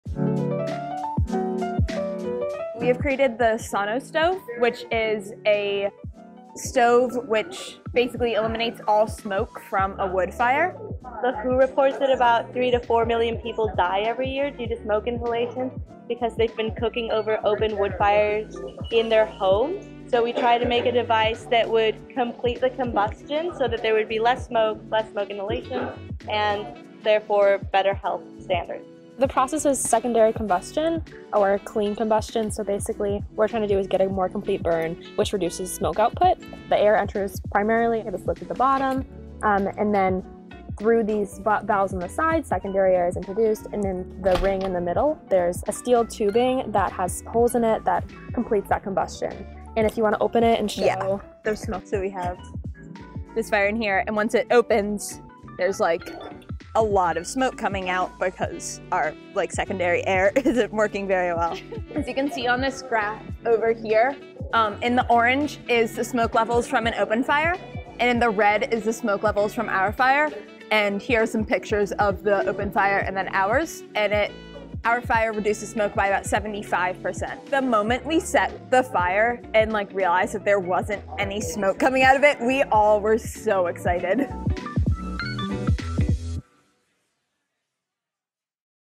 Intro to Engineering Project: SanoStove
A team of Dartmouth Engineering undergraduate students designed and built "SanoStove" for their final project in ENGS 21: Introduction to Engineering. SanoStove is a smokeless stove to reduce dangerous cooking conditions for communities that lack access to electricity. Team members: Margaret Frazier '25, Sophie Goldberg '25, Abby Hughes '25, Sara Magdalena Gomez '25, and Alda Zeneli '25; MShop instructor: Joe Poissant; TA: Jhujhar Sarna. This project received the Phillip R. Jackson Award, given each term to the group with the best overall performance in ENGS 21.